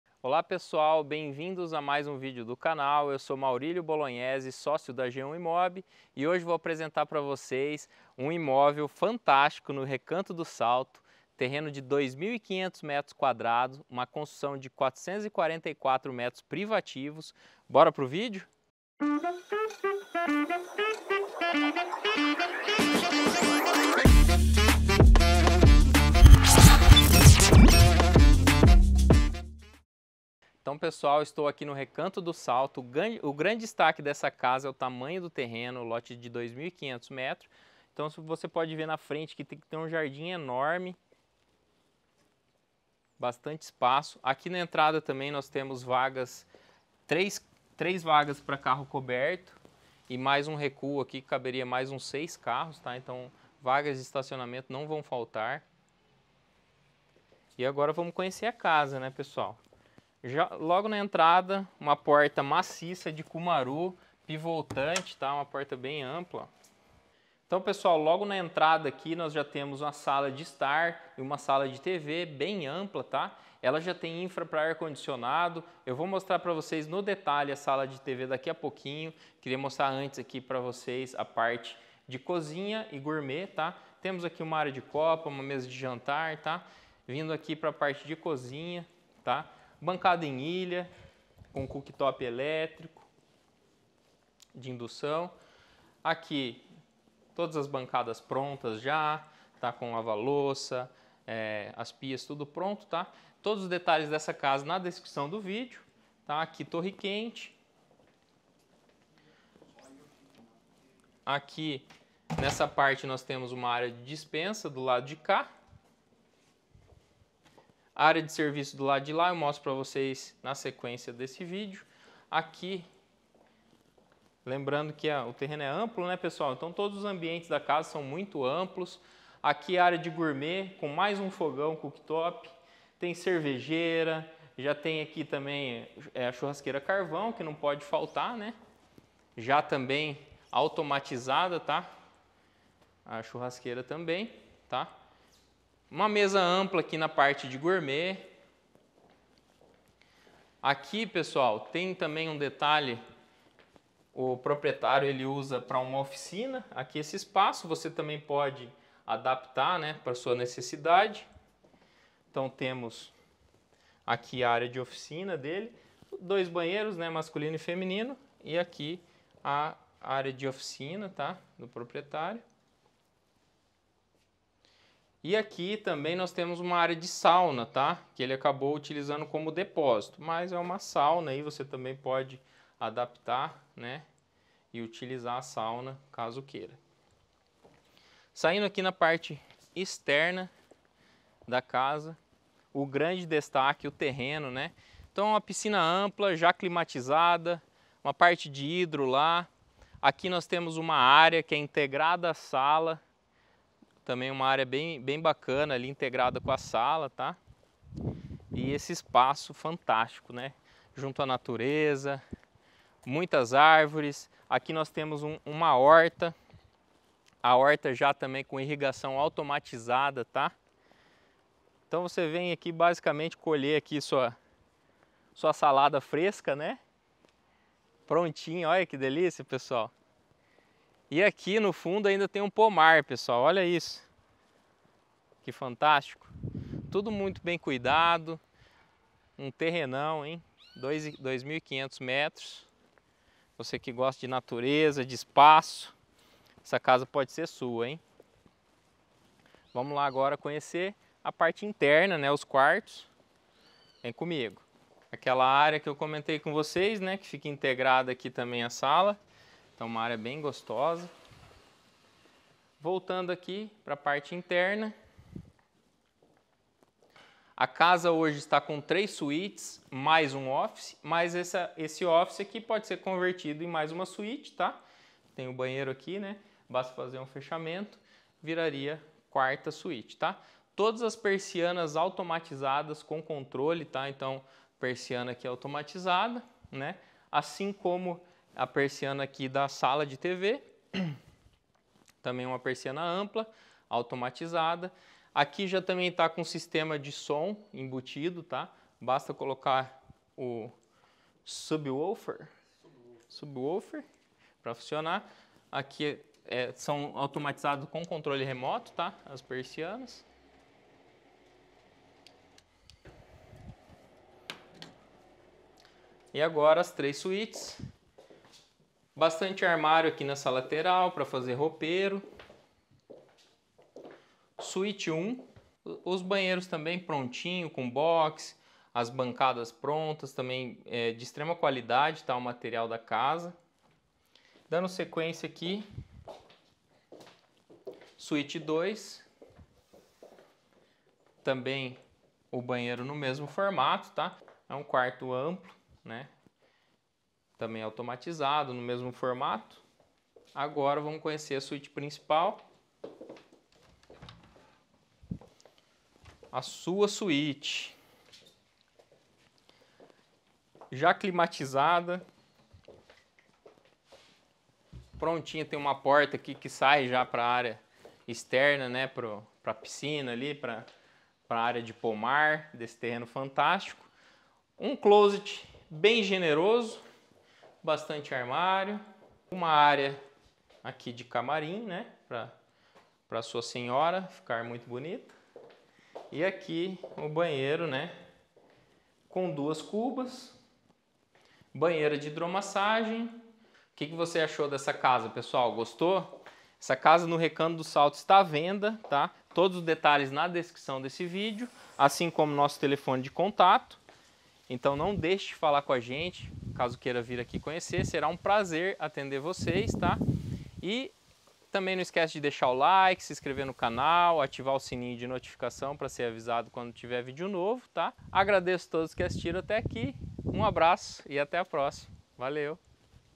[VENDIDO] Casa térrea no condomínio Recanto do Salto em Londrina. | Maurilio Bolonhesi
Casa Térrea no condomínio Recanto do Salto, terreno de 2.502,06m² de terreno e 444,44m² de construção, divididos em Salas 3 ambientes integradas, pé direito duplo em quase toda a área social, lavabo, 4 suítes, sendo 1 máster com closet e hidro, ateliê, oficina, depósito, lavanderia planejada com wc serviço, cozinha planejada e copa, cristaleira, despensa, espaço gourmet, ambientes completos em armários, persianas blackout automatizadas, piscina com ofurô aquecida, energia fotovoltaica, aquecimento solar, pomar formado com 15 tipos de frutas, horta com estufa e irrigação automática, poço artesiano individual, jardim e paisagismo em todo o imóvel.

Quer saber mais ou agendar uma visita? entre em contato comigo!